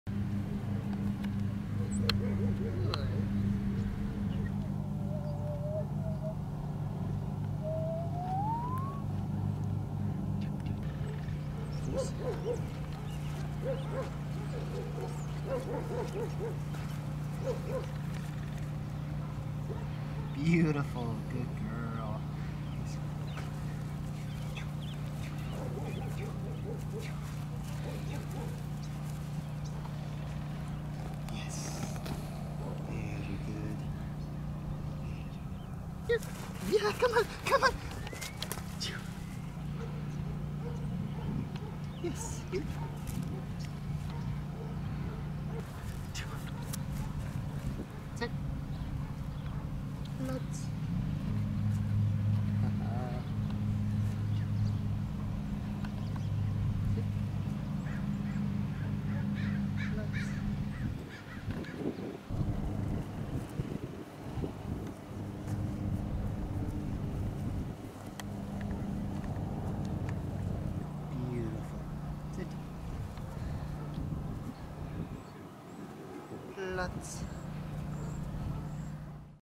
Kraftwerk K9 German Shepherd attentive & ready at 9 weeks old!
Shows natural guide-ability with excellent desire to please the handler.